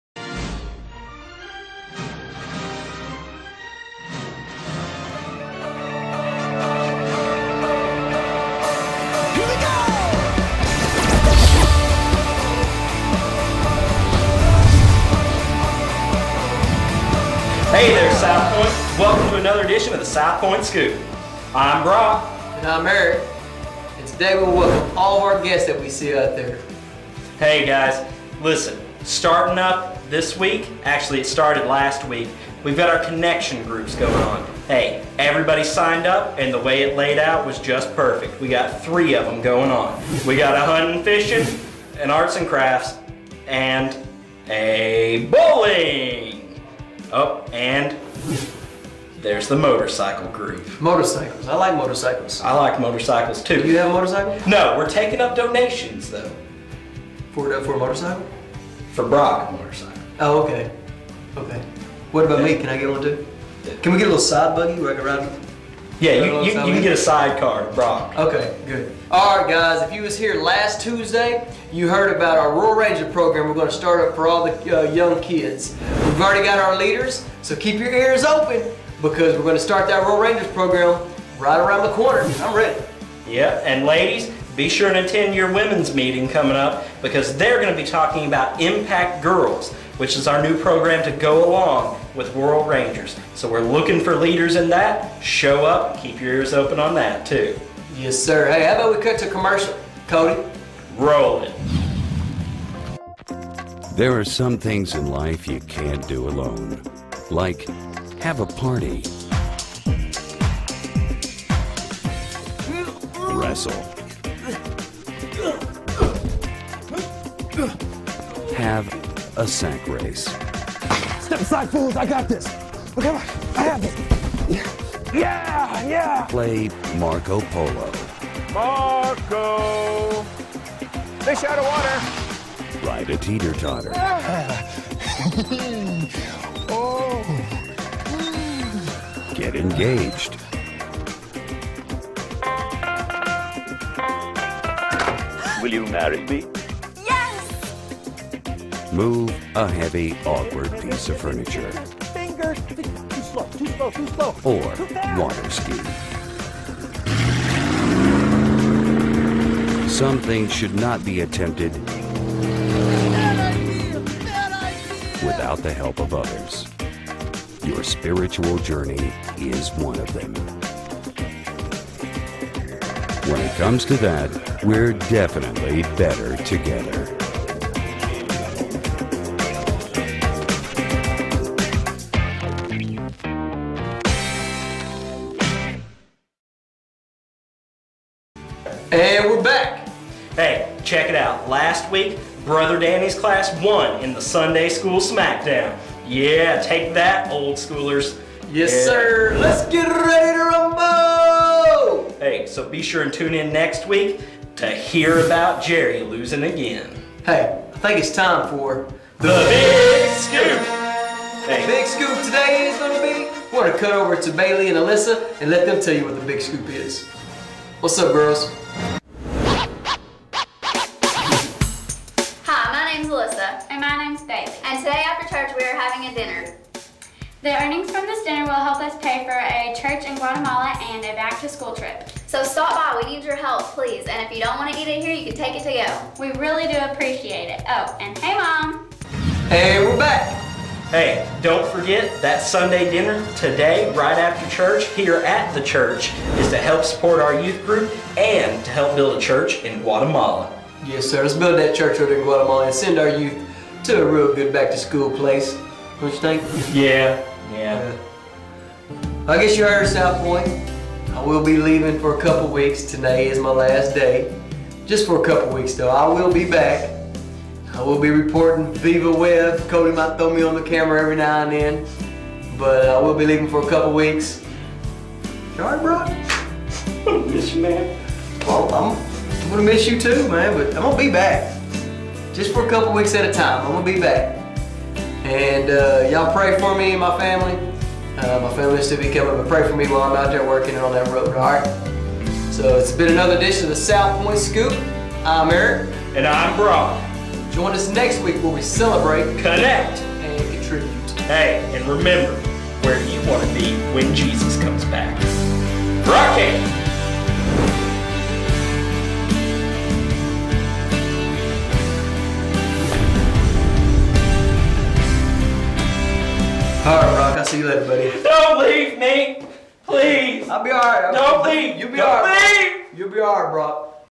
August 29 Southpointe Scoop from Southpointe Church
This week's Southpointe Scoop featuring Eric Atkinson and Brock Campbell.!